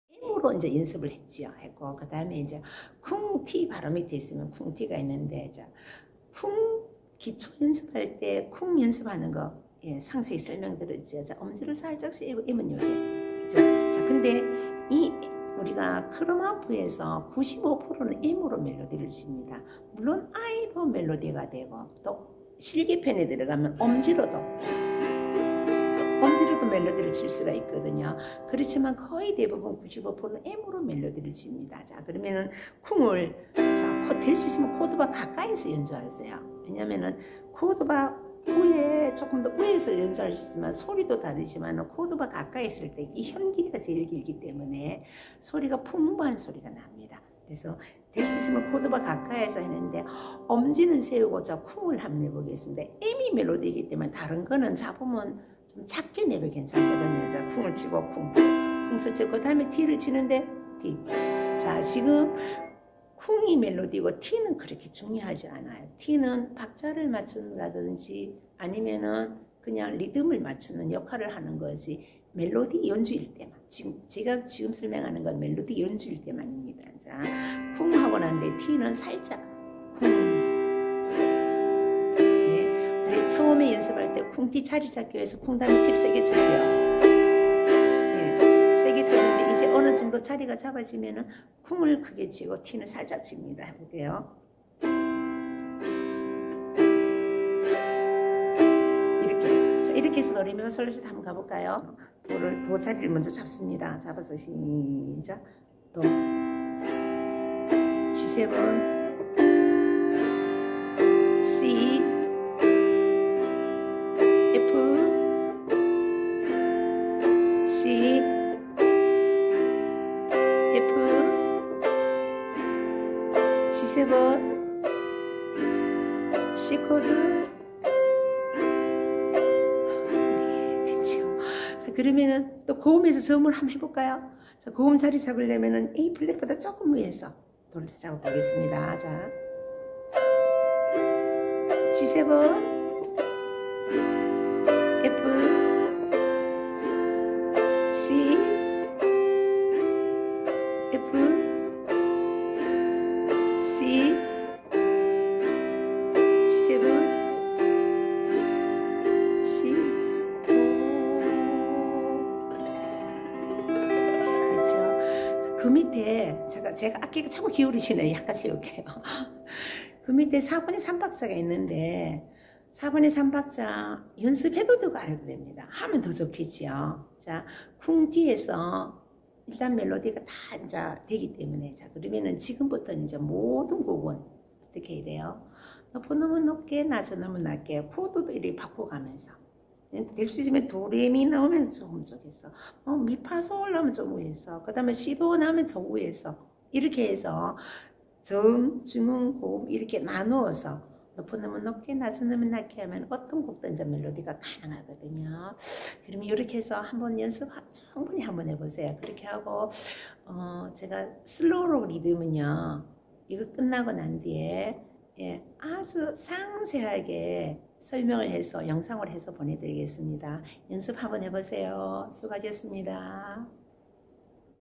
크로마하프 기초배우기 계명연습(2) 쿵티아이엠 크로마하프교본 기초편 45쪽 설명
◾ 크로마하프 기초 강좌 계명연습 (2)
◾ 쿵티 아이엠  교본 기초편  45쪽 현과계명
◾크로마하프 기초 강습 문의
◾상담 문의처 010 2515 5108
◾크로마하프 악기 문의